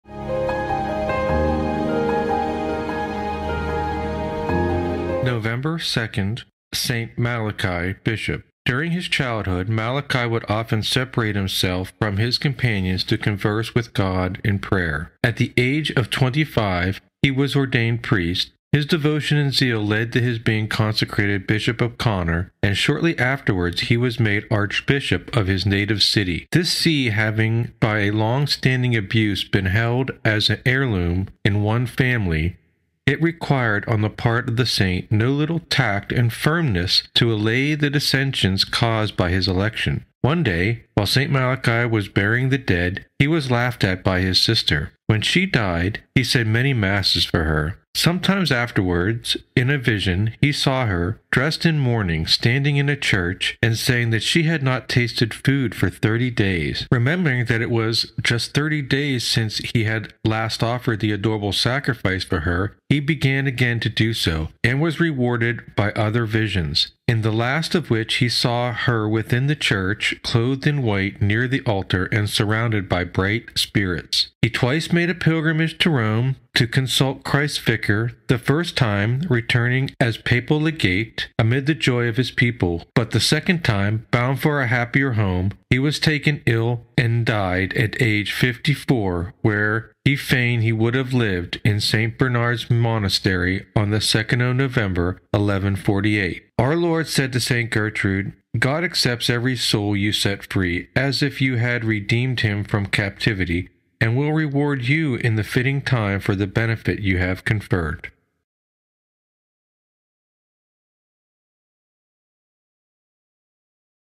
Saint Malachi, Bishop - November 2nd
Born: 1094
Died: 2 November 1148

Canonized:
1190 by Pope Clement III;

Patronage:
archdiocese of Armagh, diocese of Down and Connor

Butler, Alban. Fr., "NOVEMBER." In "Lives of the Saints: for every day of the year"."November 2 - ST. MALACHI, Bishop".,357, North Carolina: TAN Books, 2012.